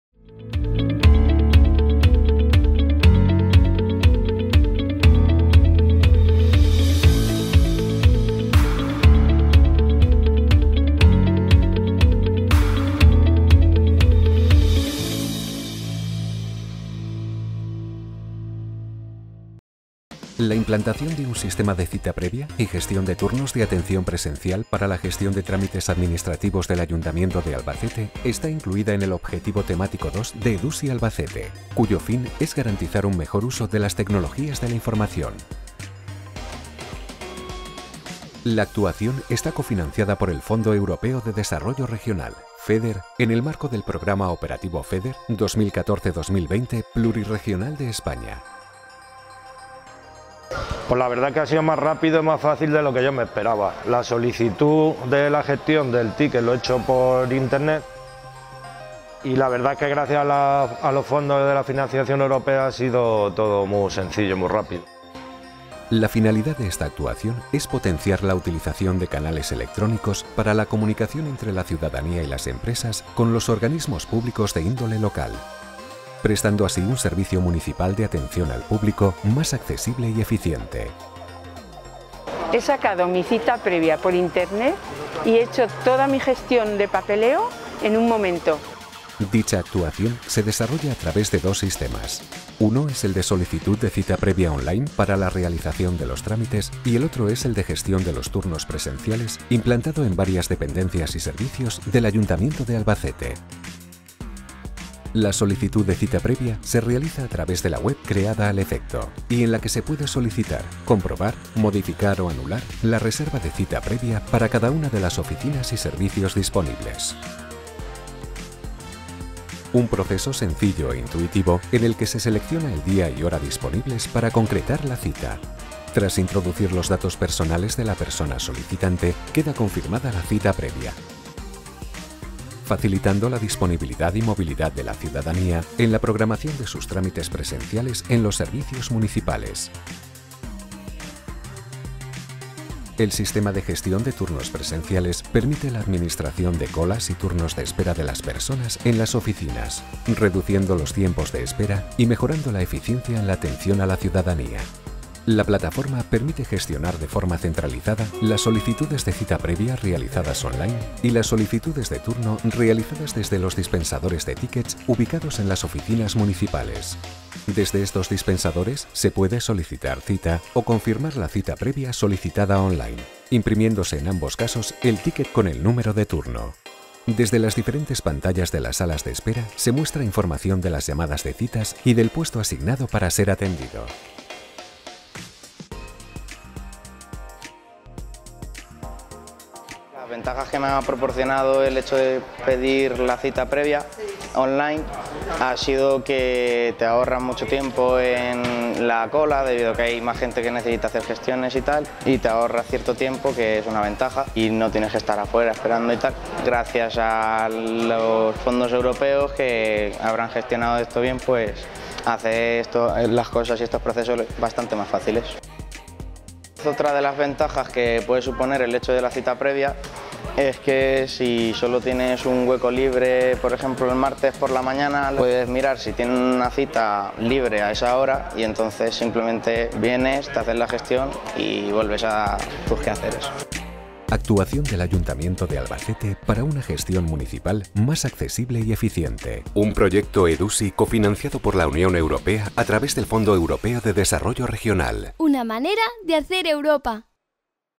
Implantación de un sistema de cita previa y gestión de turnos de atención presencial
Implantación de un sistema de cita previa y gestión de turnos de atención presencial para la gestión de trámites administrativos del Ayuntamiento de Albacete